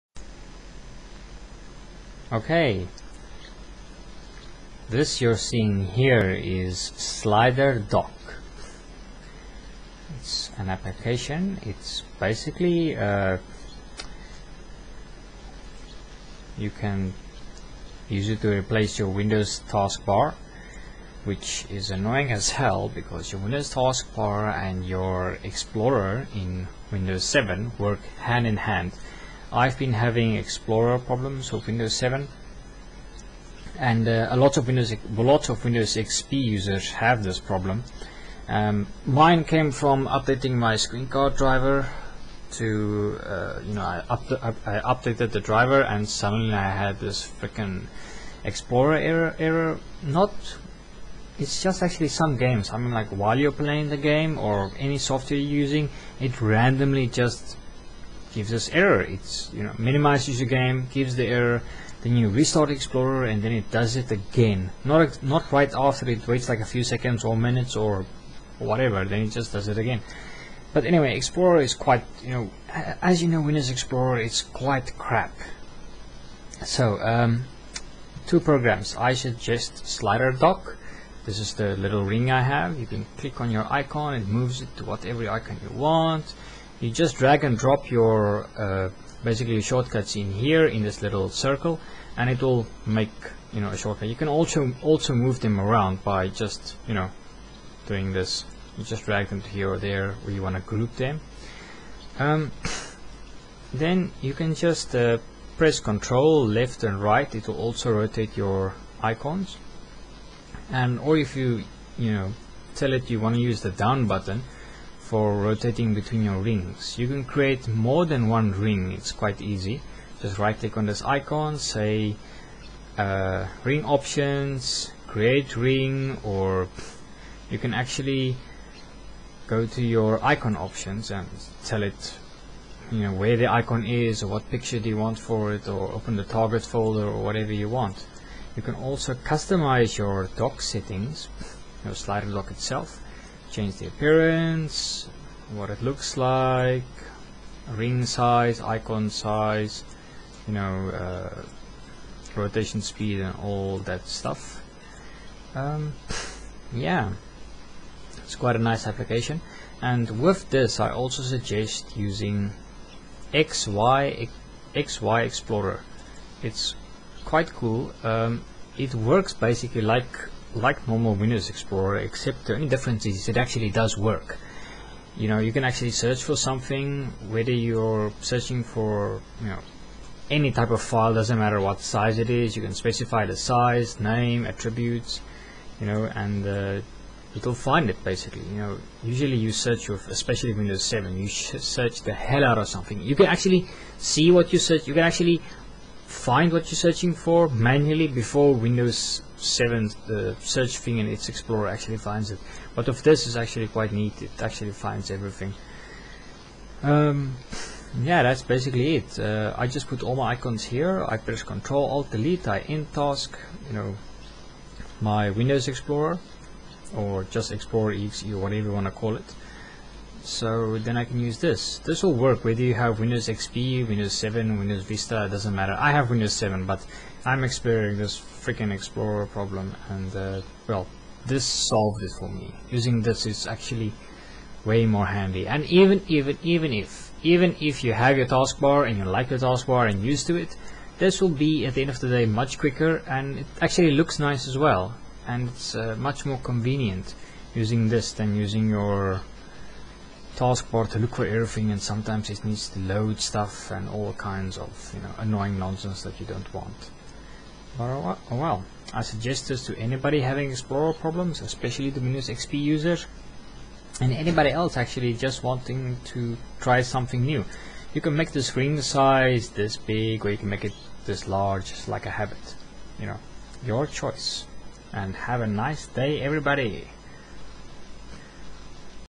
Slider dock and xyexplorer explorer replacement
I recommend this to anyone having explorer issues and taskbar problems
Windows explorer replacement
xyexplorer and slider dock :D
Oh and by the way get the portable zipped version = its easy to use and you can just unzip it anywhere and you can also tell it to startup with windows :D